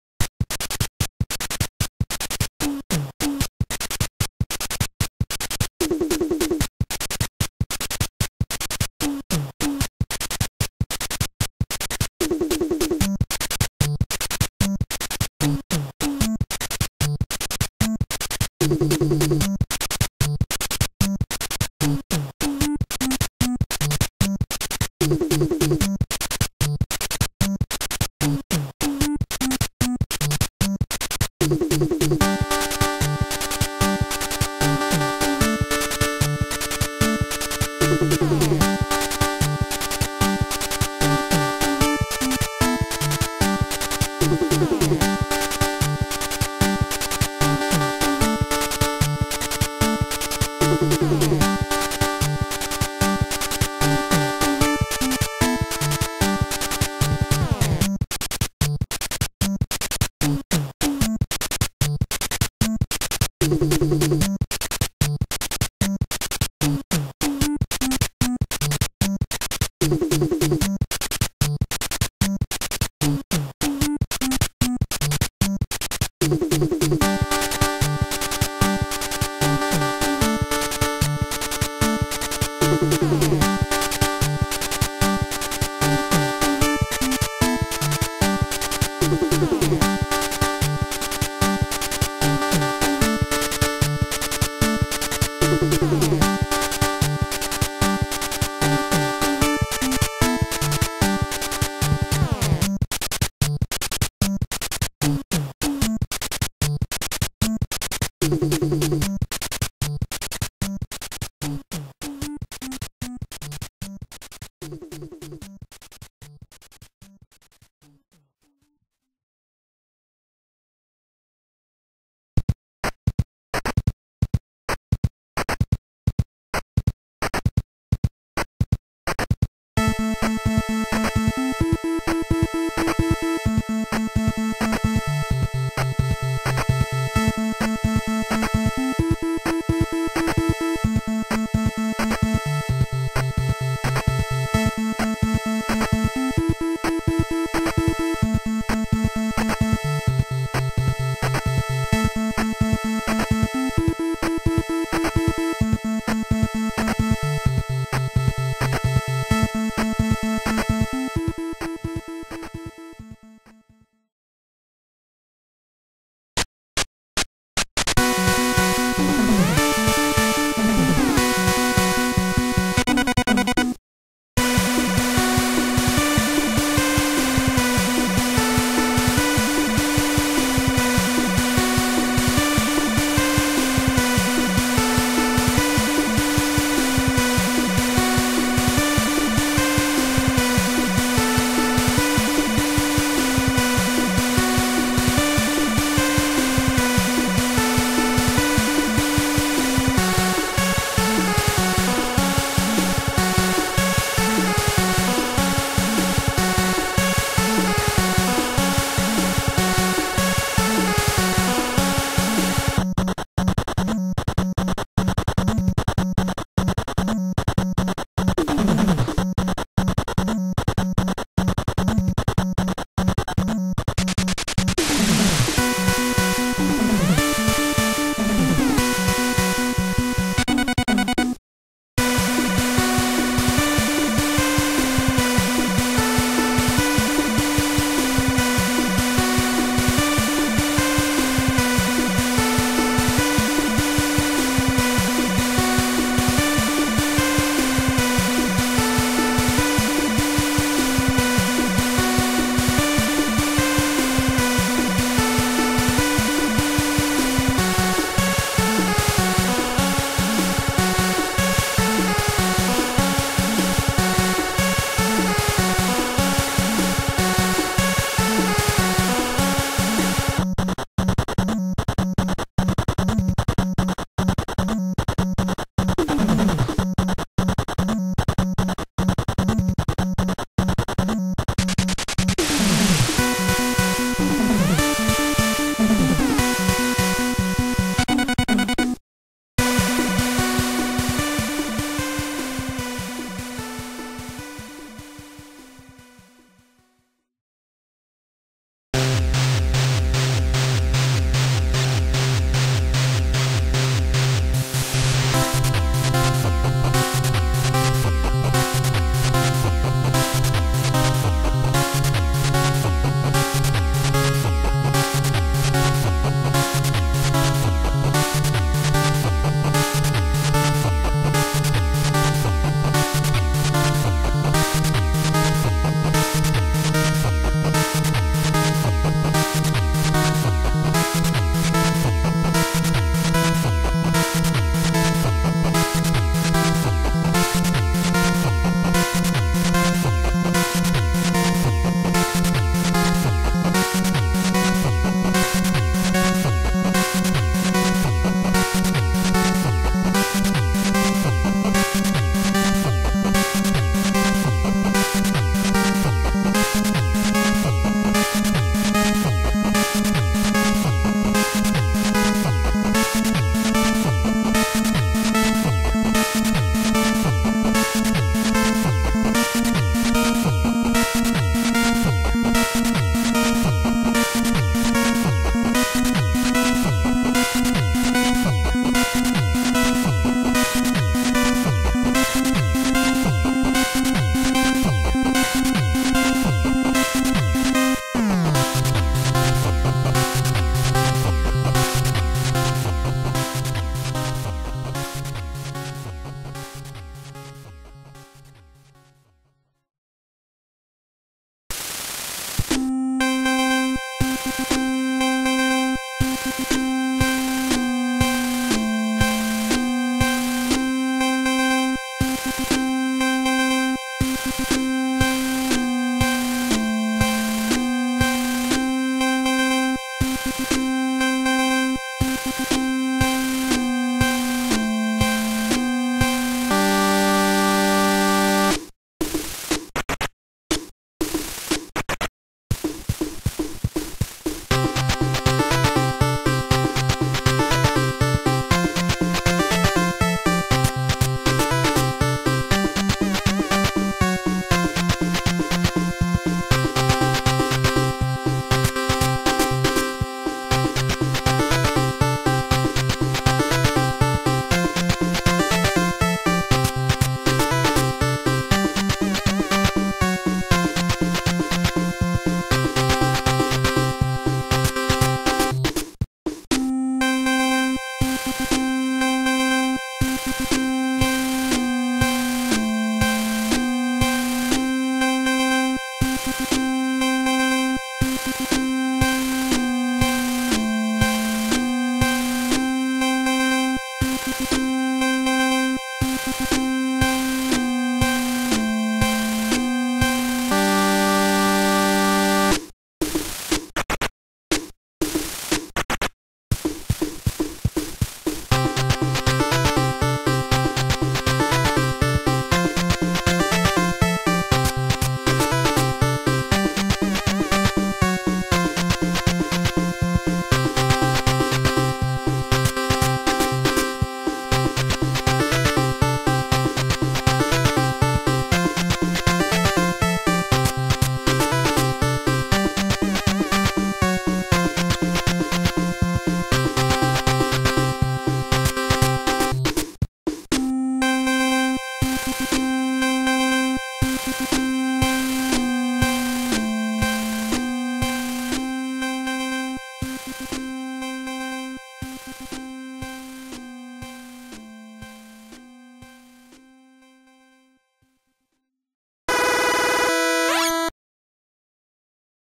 8-Bit Originals Collection
I recently had to do a math project where I had to record myself. To make it less dull and educational, I recorded few songs in 8-Bit to liven it up. One of them wasn't originally for the project, but I couldn't use it for anything else.